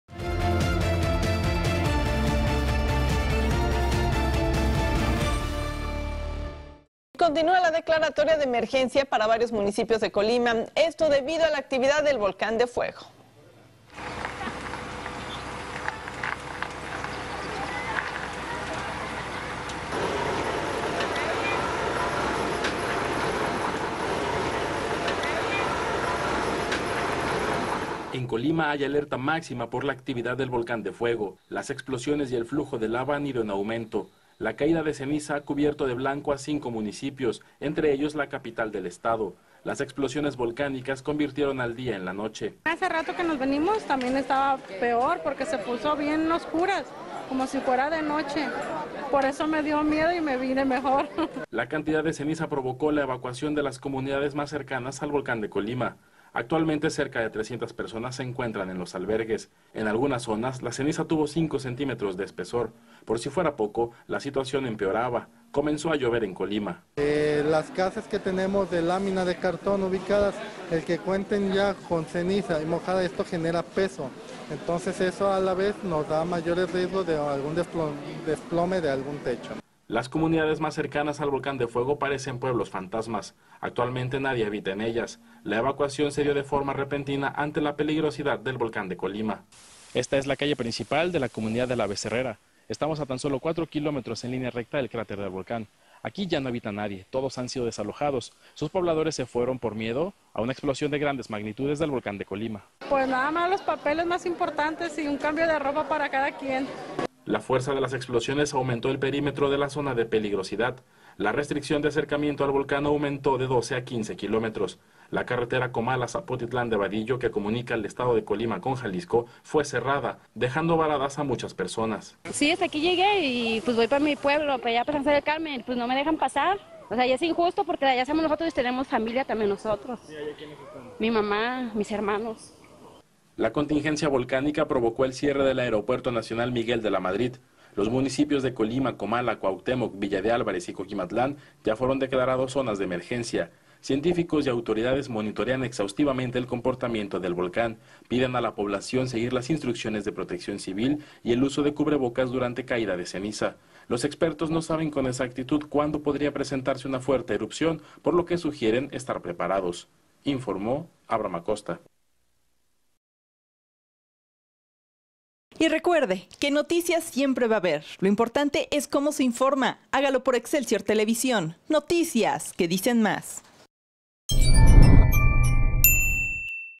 Volcán de Fuego mantiene en alerta a Colima / Vianey Esquinca
La Segob emitió una declaratoria de emergencia para cinco estaods de Colima luego de la erupción volcánica.

13 de julio 2015

Conductor: Vianney Esquinca.
Horario: Lunes a Viernes @05:30-08:30